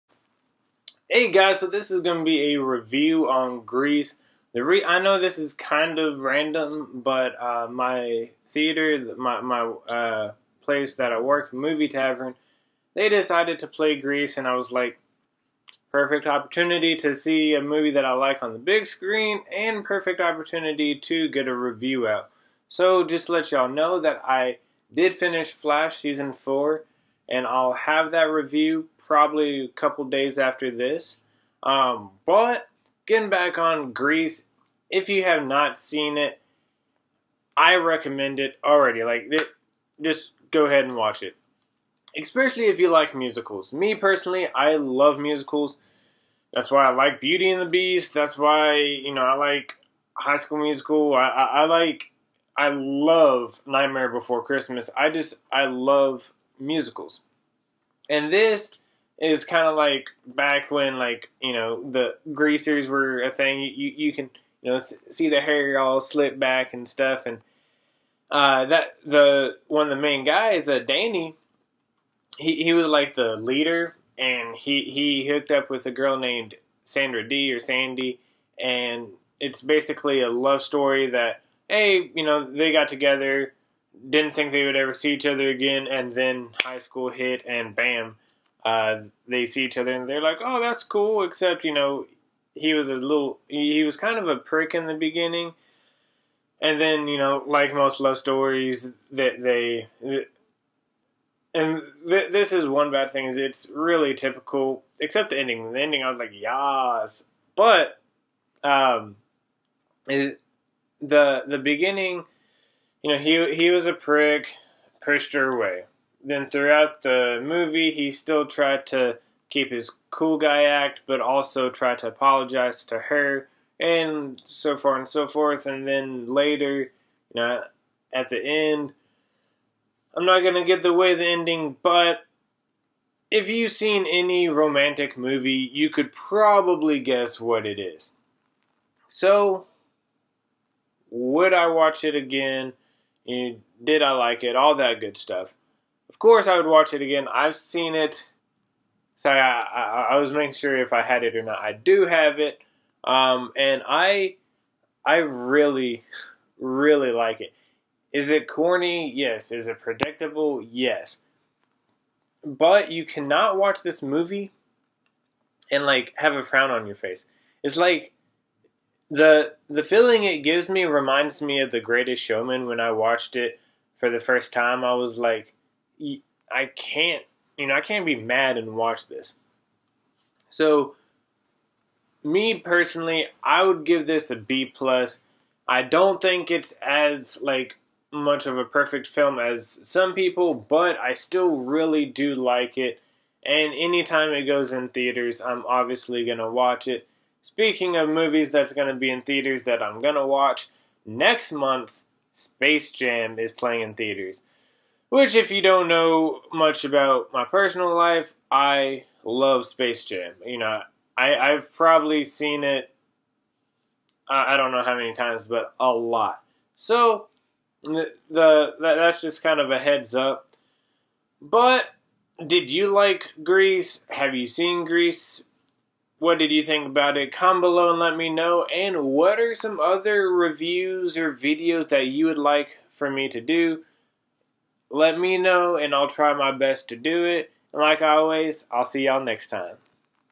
Grease Movie Review
This played at my theatre the other day and figured I would make a review.